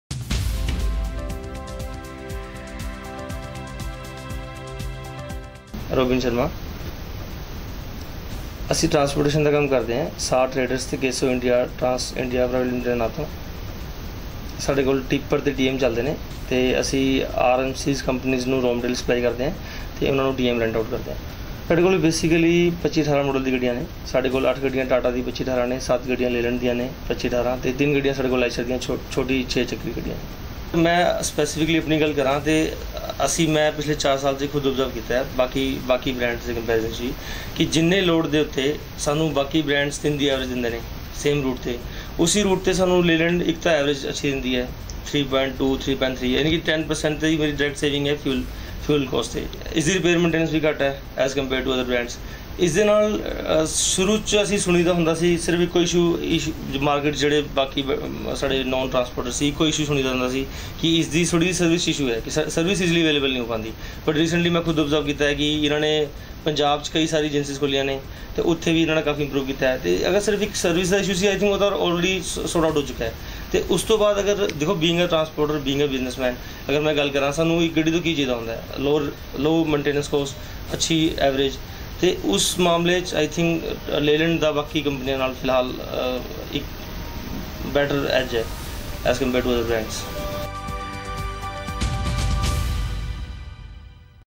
Ashok Leyland Customer Testimonial Chandigarh - U2518 Tipper
Watch the latest episode of Customer Testimonial from Chandigarh for Ashok Leyland's U2518 Tipper truck.